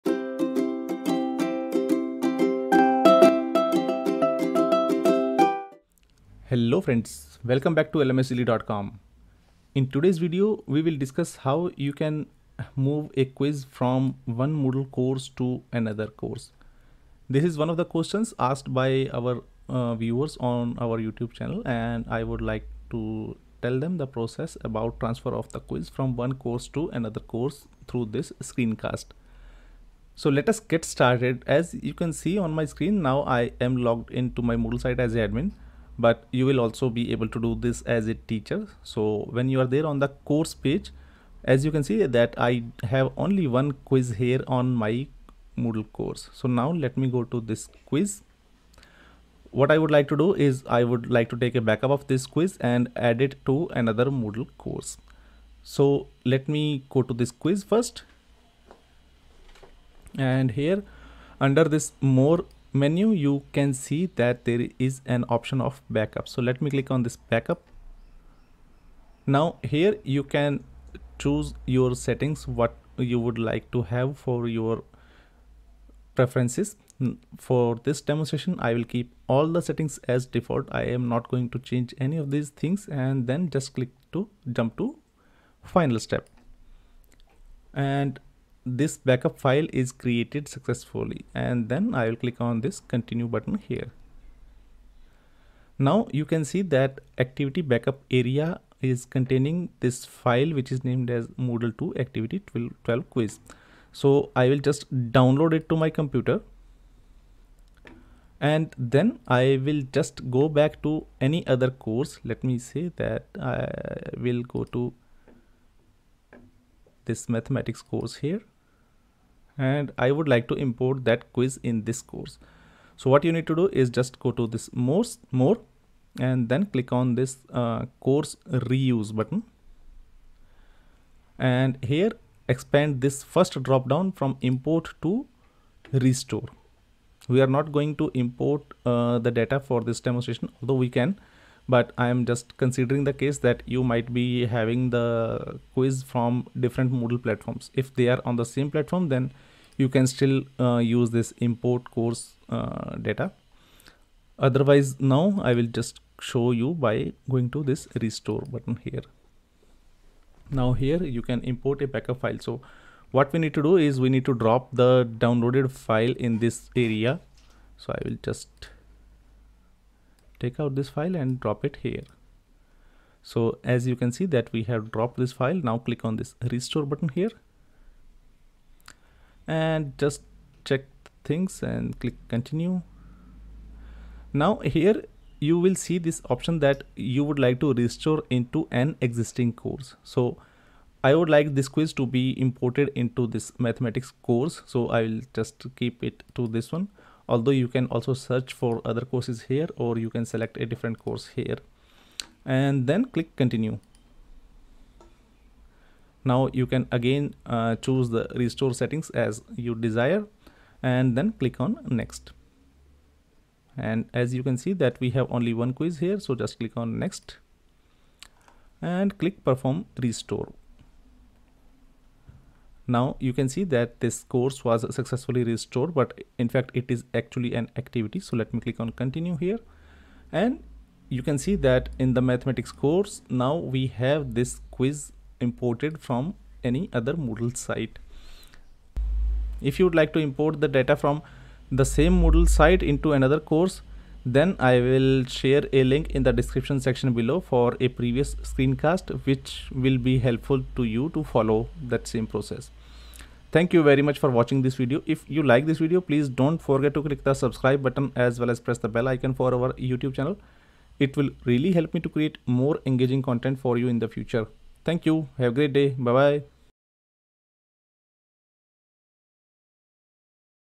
How to Import Quiz from Moodle course to other course
If you’re an educator or administrator using Moodle, you might be looking to reuse the course material on your Moodle site. In this screencast we will see how you can import a quiz from One Moodle course to Another Moodle course (On a different Moodle site)

The process is similar to the course backup & restore but here we need to backup only the Quiz activity and restore that in our Moodle course.

If you are looking to import the quiz on same Moodle site, I will recommend that you should go through this screencast.